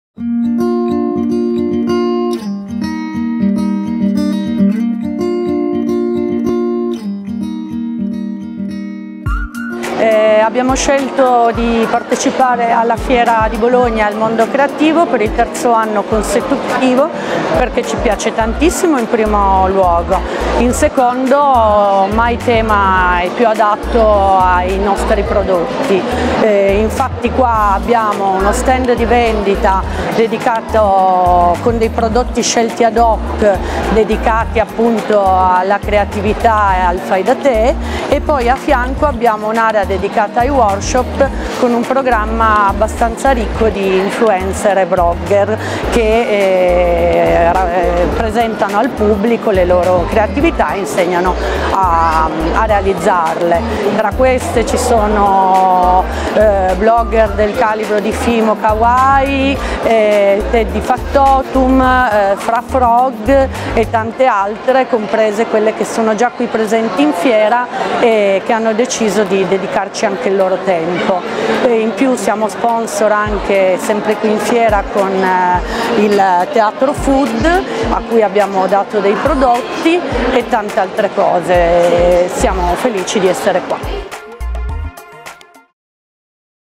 Il Mondo Creativo 2018 | Margherita Dioniso, Flying Tiger Copenhagen
Intervista a Margherita Dioniso, Flying Tiger Copenhagen, in occasione della nuova edizione autunnale de Il Mondo Creativo 2018, dal 23 al 25 novembre a BolognaFiere. La manifestazione dedicata alla creatività e alla fantasia, da più di dieci anni uno degli appuntamenti italiani leader nel settore dell’handmade, ha richiamato nei padiglioni 25 e 26 dell’expo bolognese tantissimi appassionati. L’evento ha rappresentato una full immersion di shopping creativo con proposte di prodotti, novità e tendenze.. - video by Lab Video